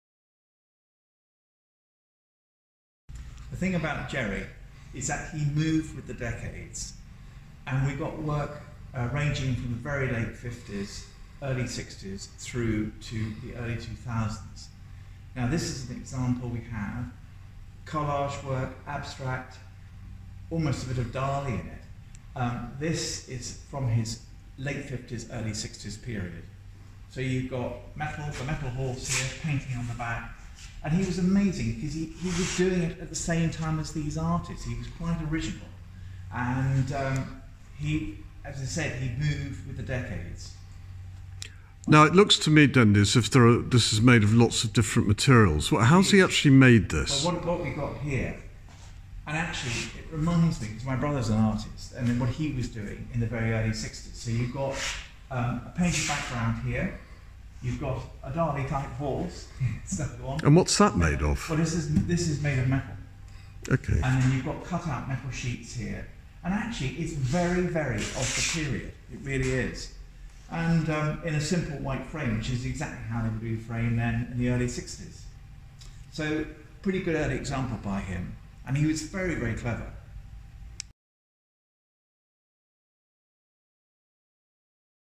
Lot 131 Abstract with Metal horse (18th May 2021 Auction)
Gerald Moore
Abstract with Metal Horse
Initialled GM, Mixed Media
121cm by 120cm
£200-300